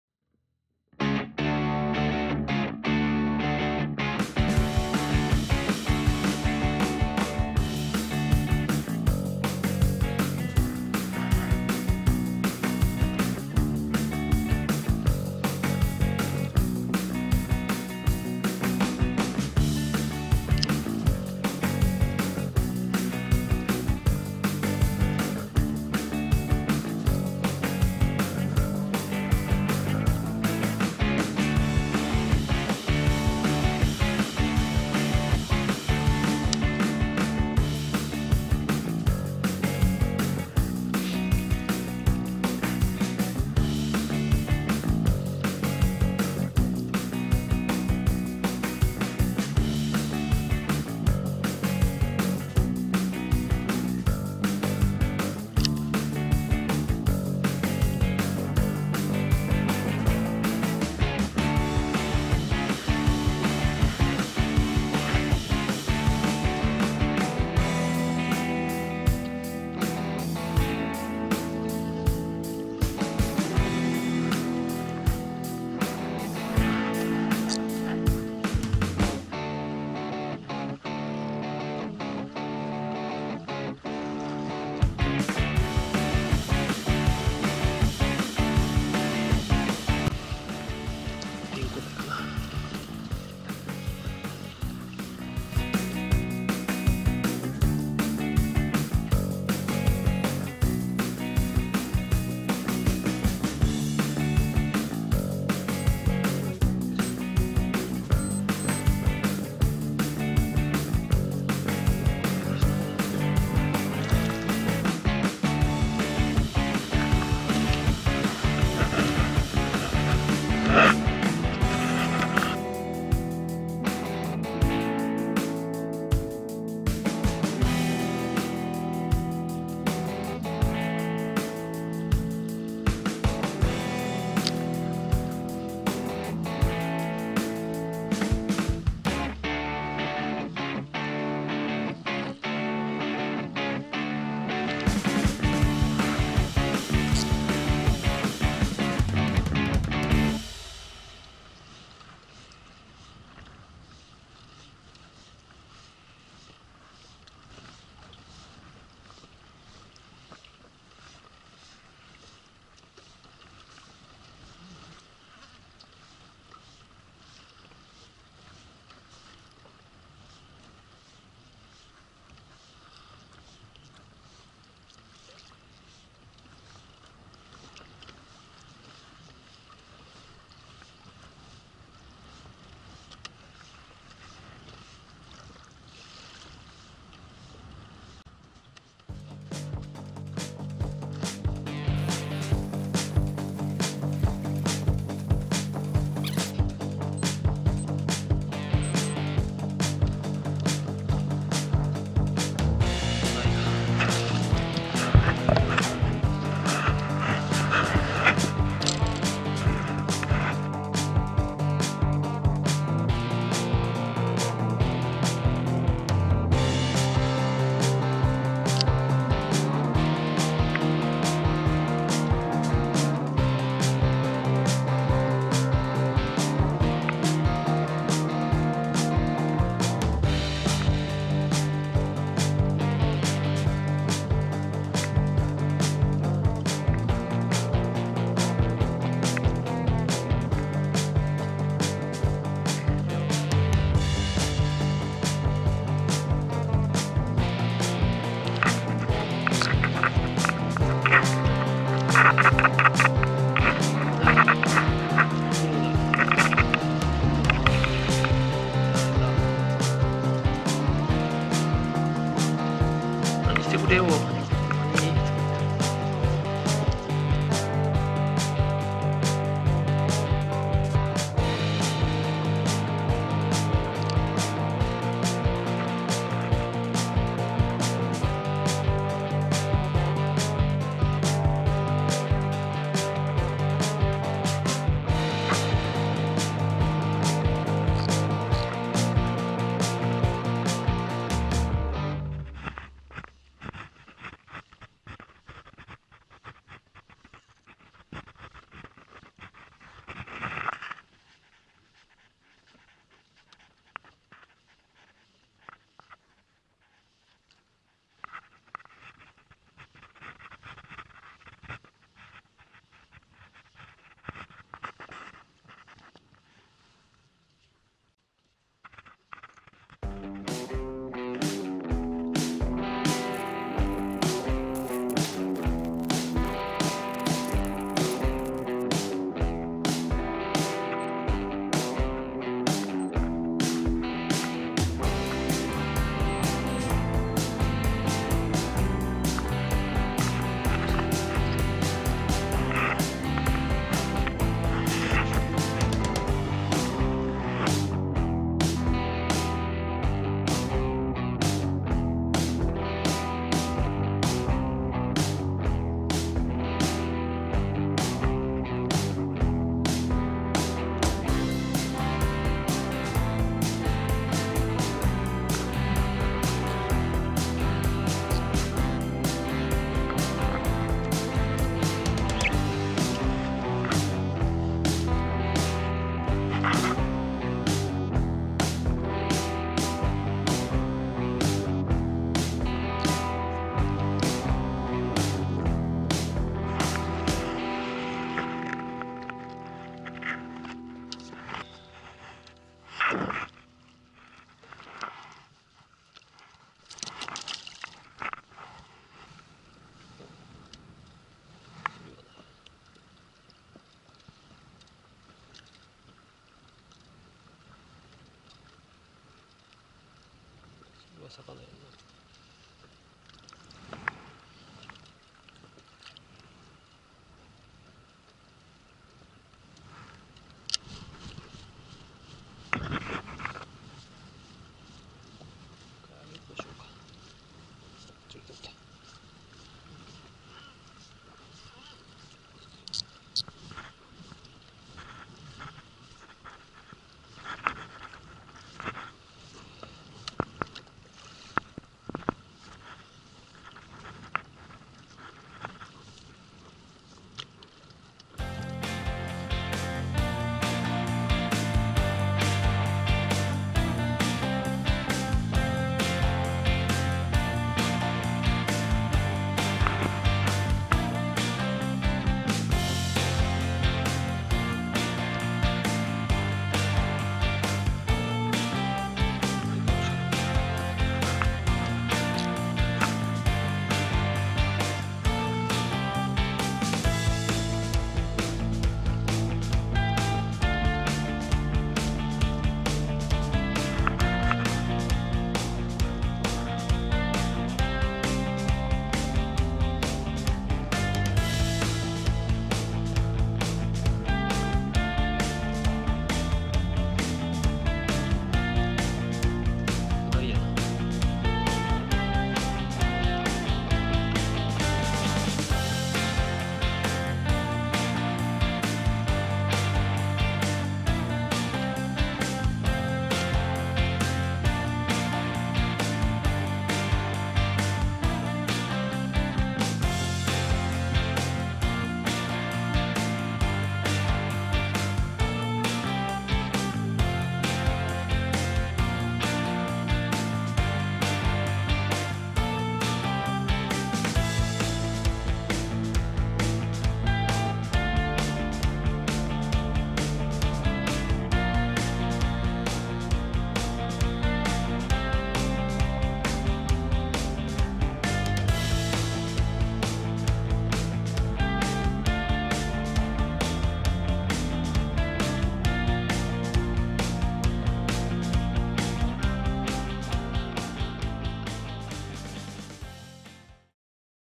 久しぶりのタイラバに本命ヒット！？サイズはともかく嬉しい獲物！？
この日は約一カ月ぶりのタイラバに挑戦。
程よい風はあるものの潮が行かず苦戦です。

釣れるのは小物ばかり・・・・

最後にヒットした魚は？　そしてその結末は？